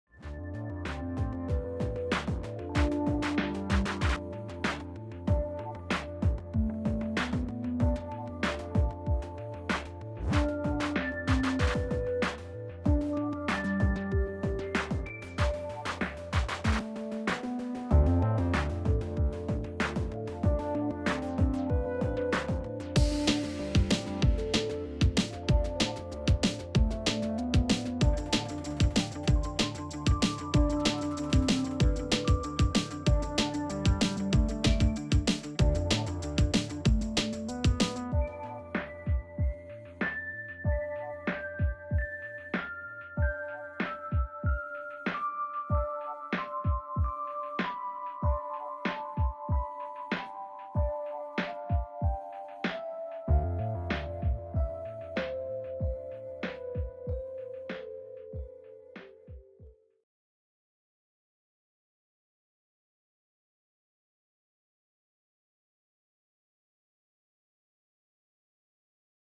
Aufgemacht: Edeka Bauerngut Zungenrotwurst 2023
Hier aufgemacht - Zungenrotwurst von Edeka Bauerngut. (03/2023)